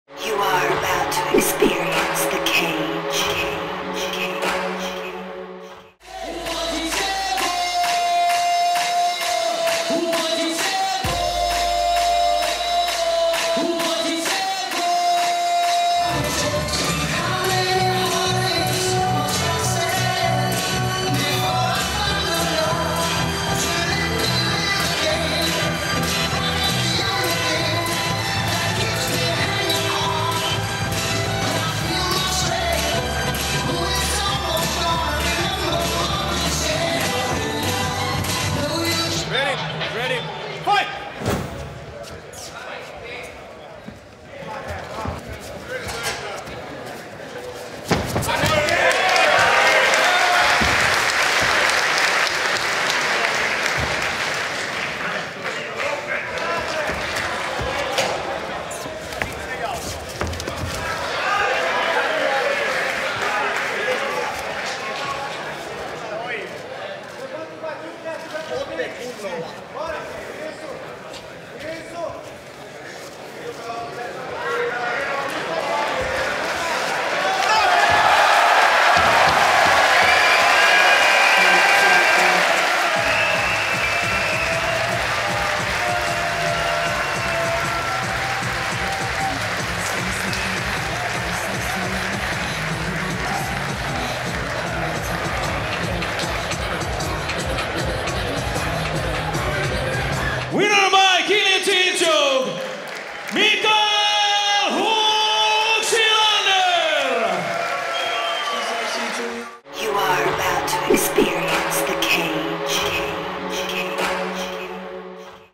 CAGE 48 Mikael Silander vs Carlos Eduardo De Azevedo Fight Highlight
CAGE 48 Helsinki 7.9.2019
Mikael Silander vs Carlos Eduardo De Azevedo Fight Highlight
Next: CAGE 49 Helsinki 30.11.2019

CAGE = Mixed Martial Arts Leader of North Europe
CAGE = Vapaaottelua Pohjois-Euroopan huipulla